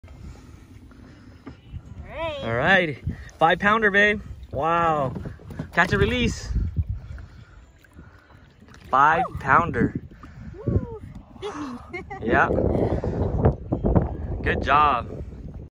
Sycamore Island 5 pounder
5 pounder caught at sycamore island 05/21/2021